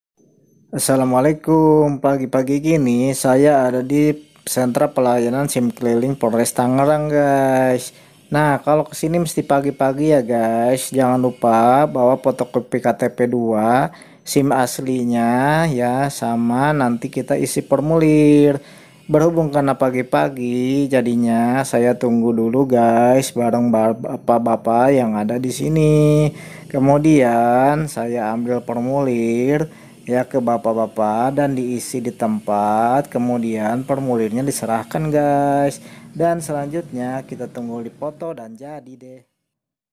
SENTRA PELAYANAN SIM KELILING POLRESTA KOTA TANGERANG
Assalamualaikum, wr.wb.
Apa kabar sobatmania kali ini saya ajak untuk datang ke sentra pelayanan SIM keliling Polresta Kota Tangerang tepatnya di Perumahan Telaga Bestari.

Adapun syarat syaratnya yaitu :
1. 2 FC KTP Elektronik
2. SIM Asli yg akan diperpanjang
3. Mengisi formulir pendaftaran
4. Menyerahkan formulir yg sudah diisi kepada petugas
5. Antri menunggu proses
6. Foto background biru
7. Bayar biaya admin Rp. 250.000
8. Ambil SIM A,B,C dll
9. Selesai